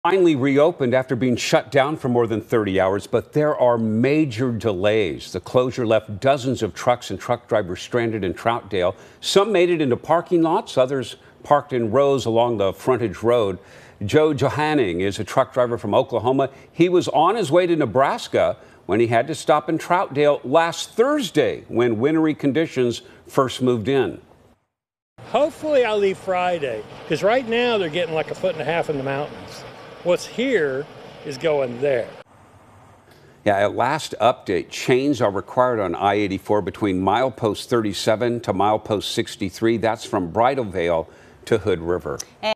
Trucks, drivers, stranded in Troutdale
I-84 in the gorge reopened after more than 30 hours. But there are still major delays.
The closure left dozens of trucks and truck drivers stranded in Troutdale.